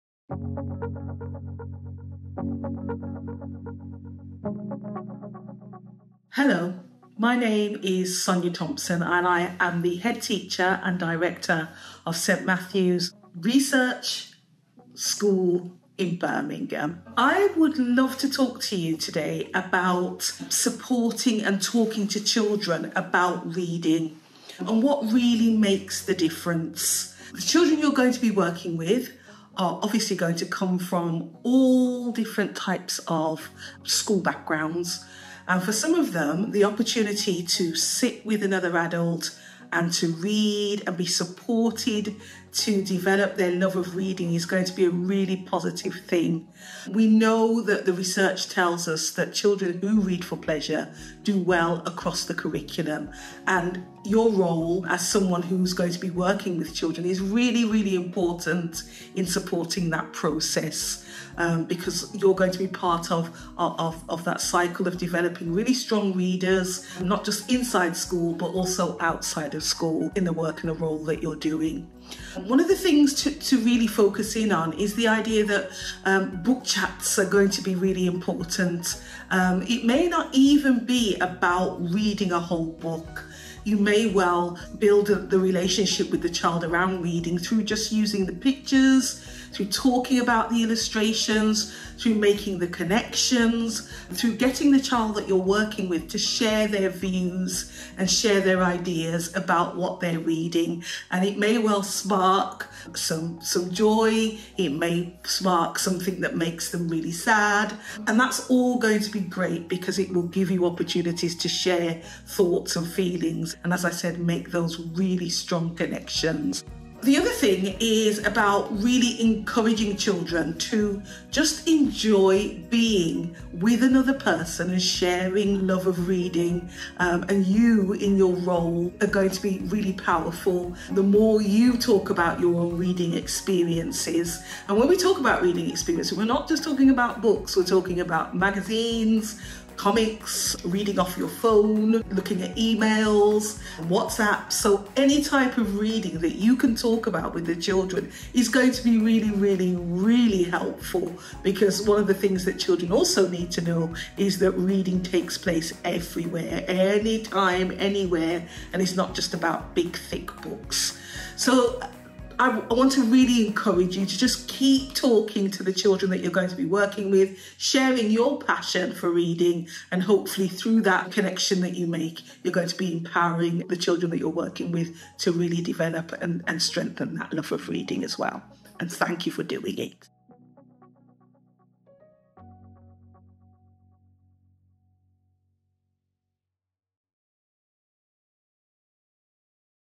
Supporting Children with Reading
This is the final video in the Celebrate Reading with HAF pack. In it,  you'll learn about:
- How Book Talk supports children with their reading
- Your role in this process